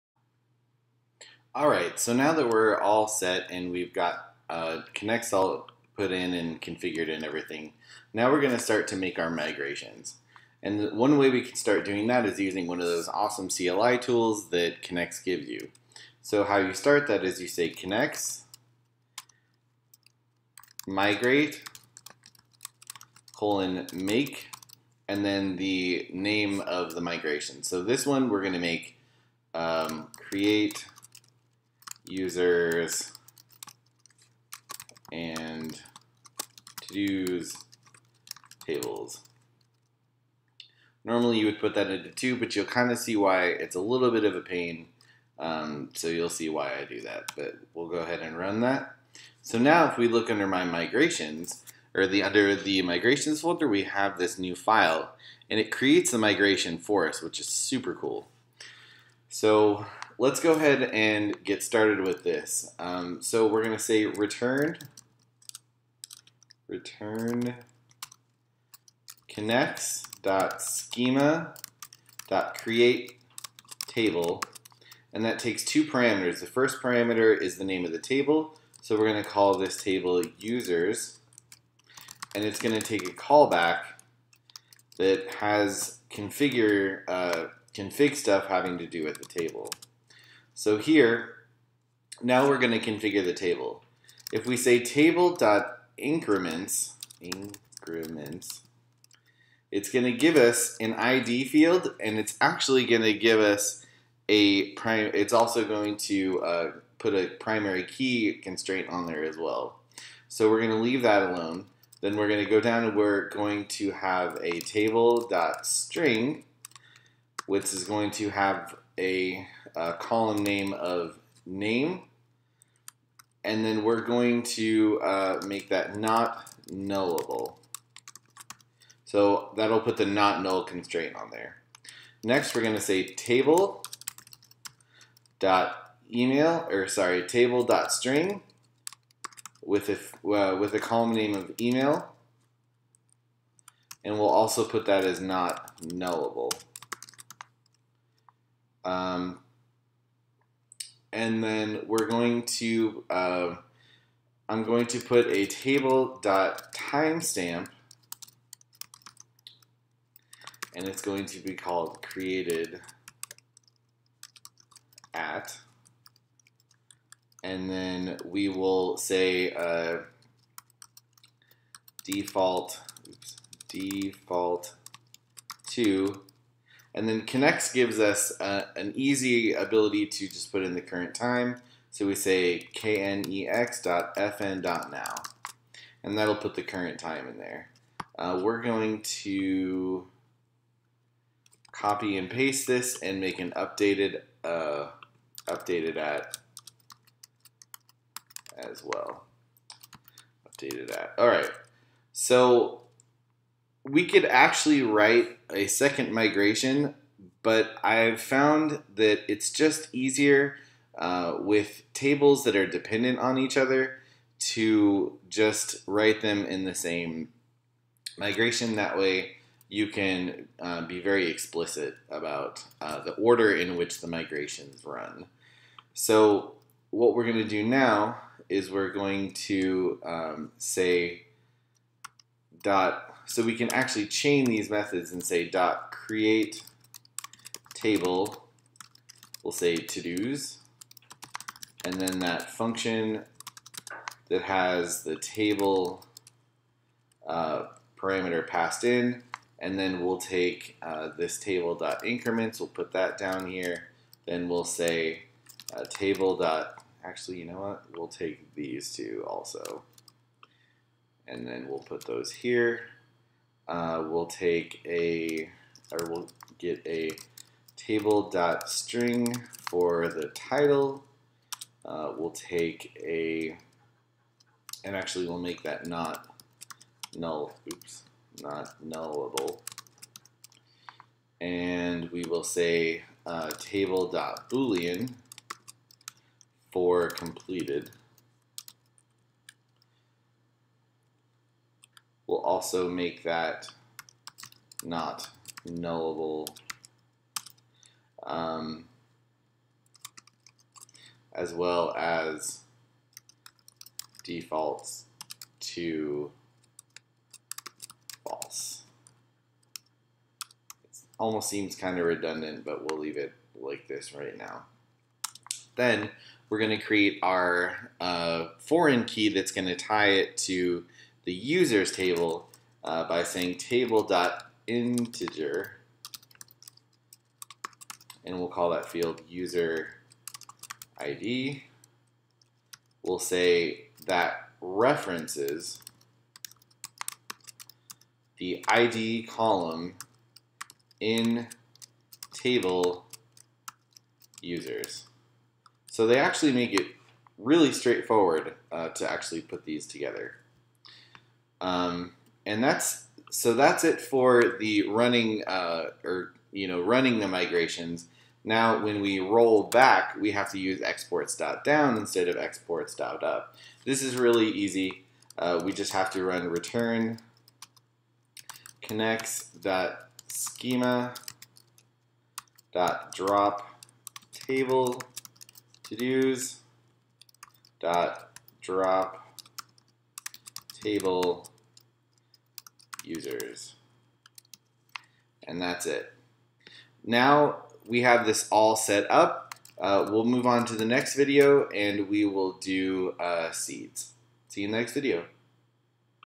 KnexJS Tutorial - 3 - Creating Migrations files
This video shows you how to create migration files for KnexJS and PostgreSQL.